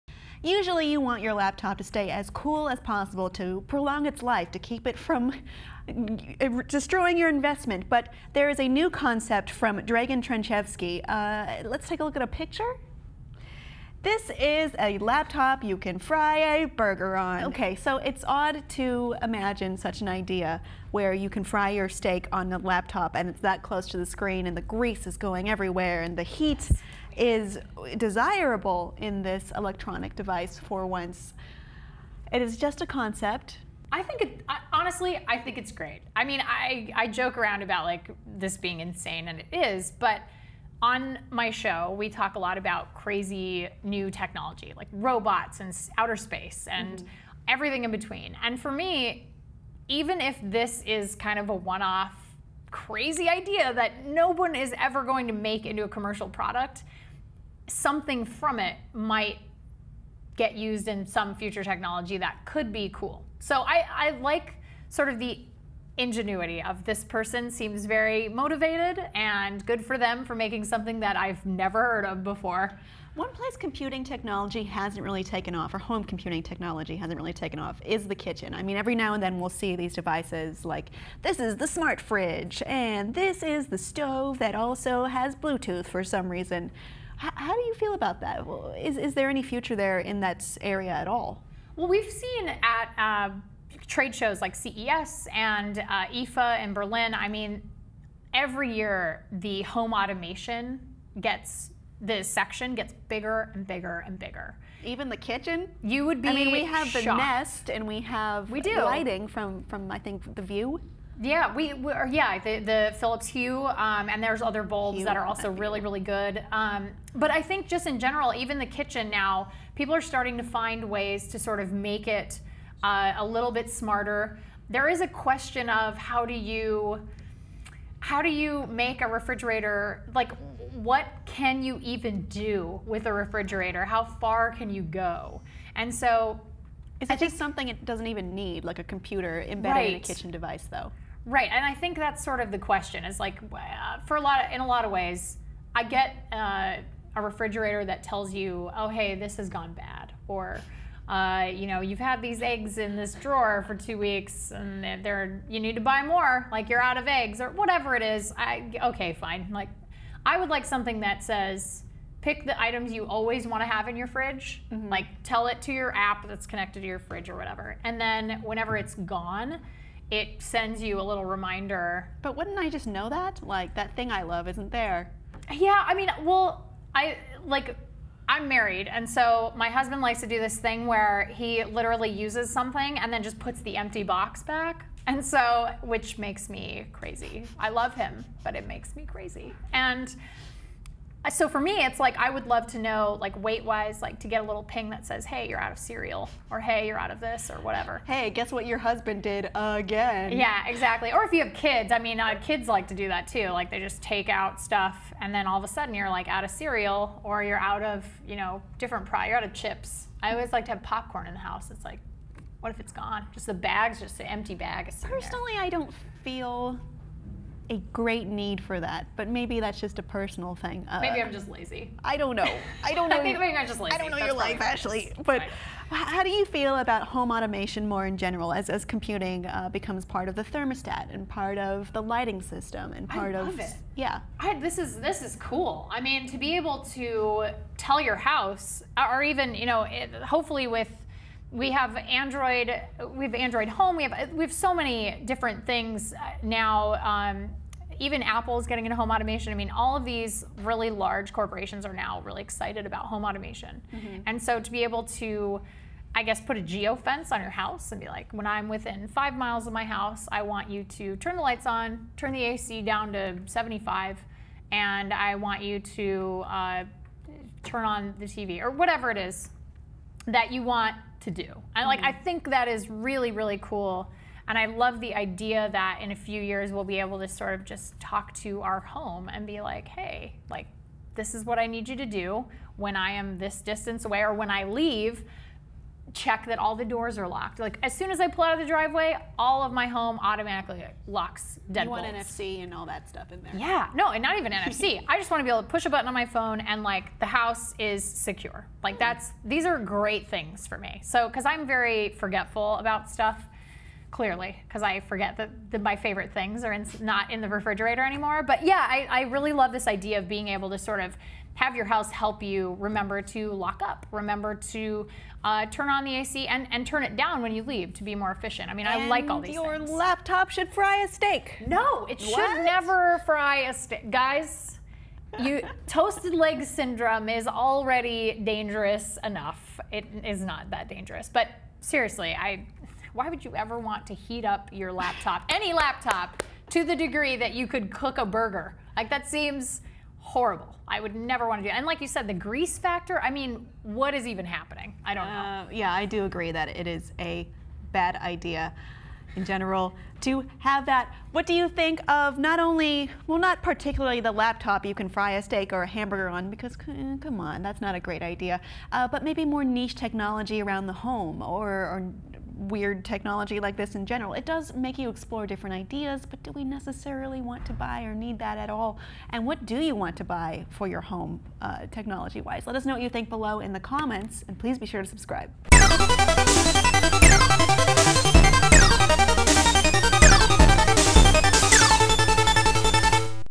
Weird Tech: The Laptop That's Also a Stove
Meet the "Electrolux Mobile Kitchen." Dragan Trenchevski designed the computerized cooktop as a concept, but he's put a lot of thought into the induction cooking device with its several pop-out cooking implements and accessories. It seems a stretch that any cook would need this device, and perhaps, most computerized kitchen gadgets in general. Is there a place for such extremely-niche devices? Kim Horcher and Ashley Esqueda (CNET Senior Editor, Host of Tomorrow Daily) discuss!